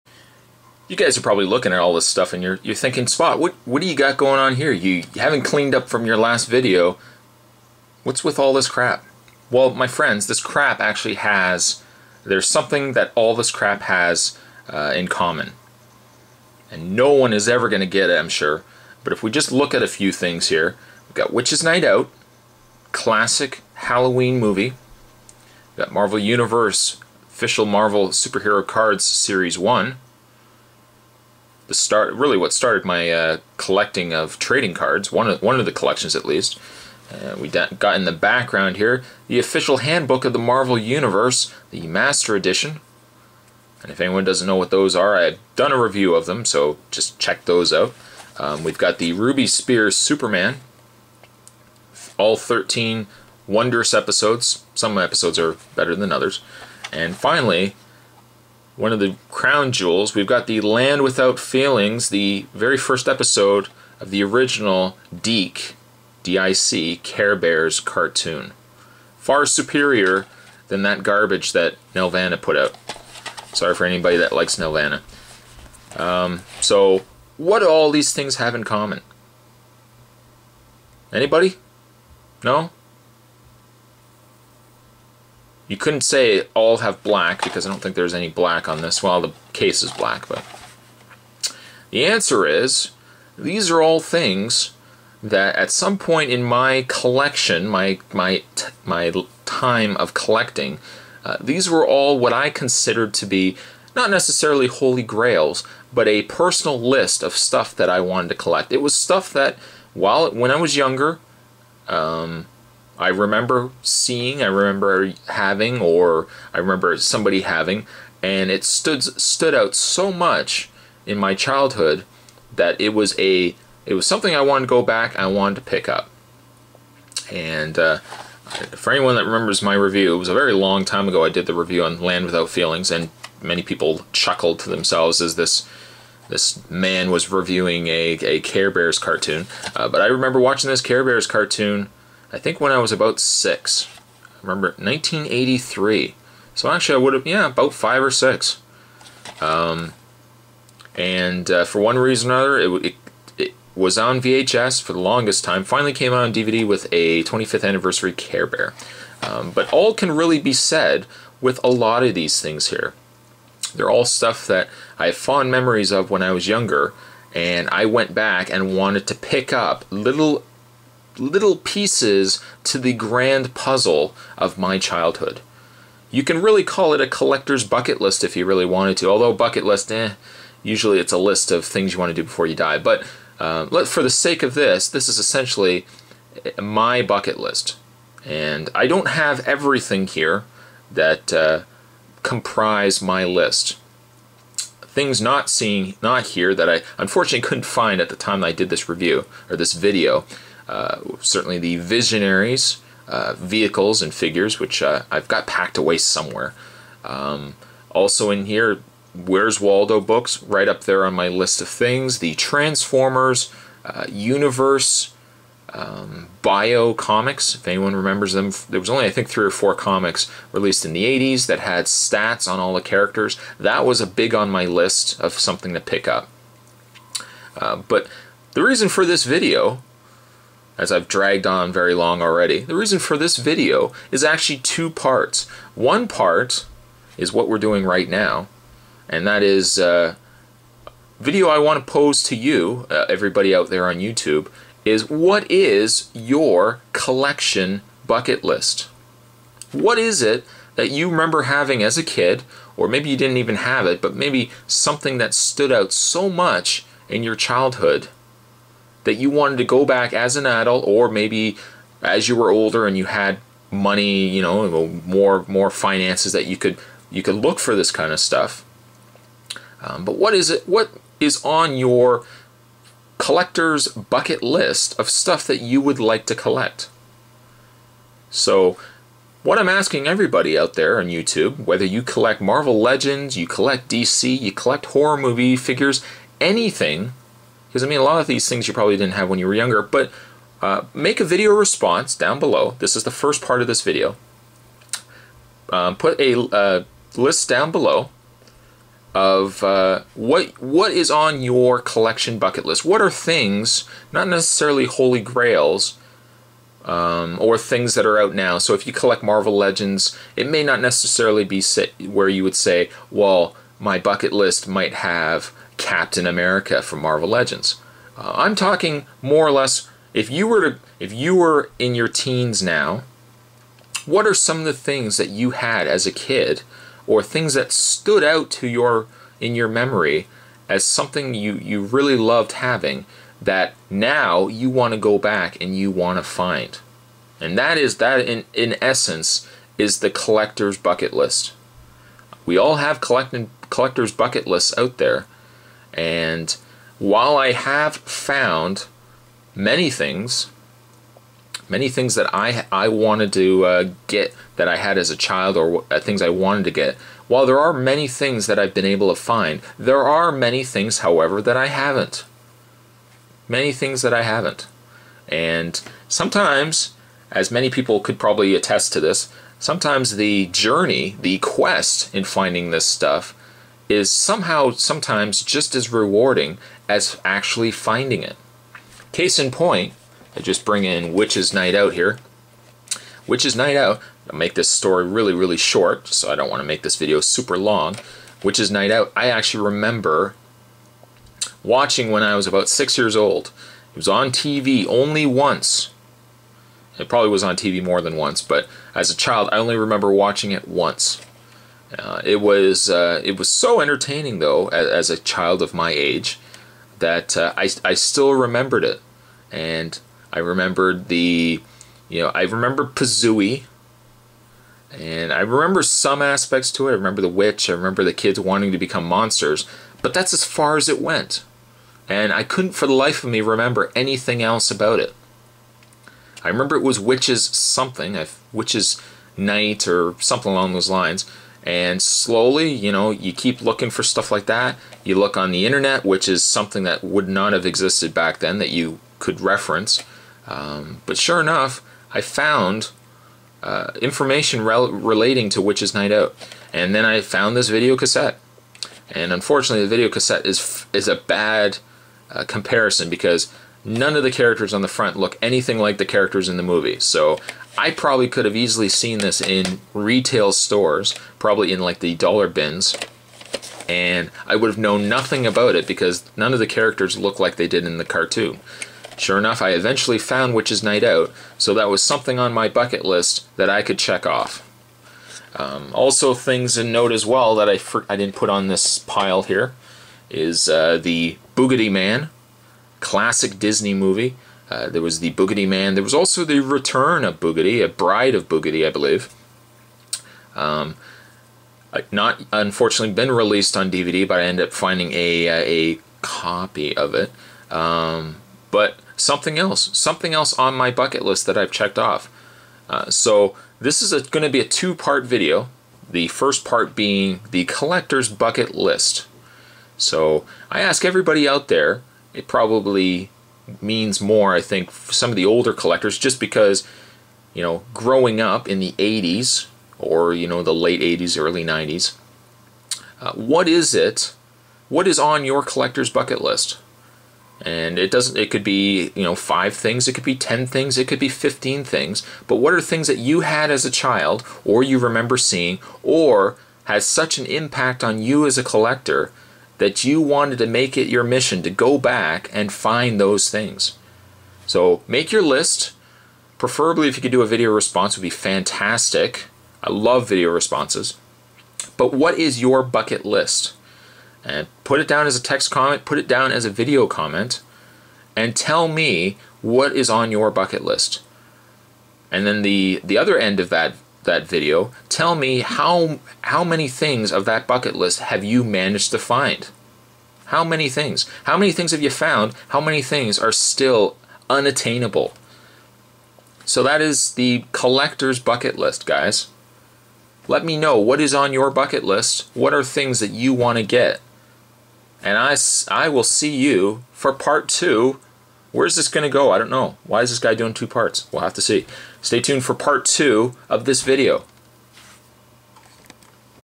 The Collector's "Bucket List"
We're going to call this little video the Collector's "Bucket list" and what is the Collector's "Bucket list" you might ask? Is there a toy or something that you had bad in the day when you were growing up? maybe it was something you always wanted like a book or a game that you remember your friend used to bring to school with him..do you have things like that where now that you're older you wish you could go back and find and add to your collection? well that's the Collector's "Bucket list".

I'm sure we all have them, those little things that by getting them would bring back a fond childhood memory, what's on your Collector's "Bucket list"?, comment down below or make a video response to this video listing your own Collector's "Bucket list", maybe tell a story or two behind why you wanted to get it now or what it meant to you as a kid and tell me if you've managed to find them or which ones have remained un found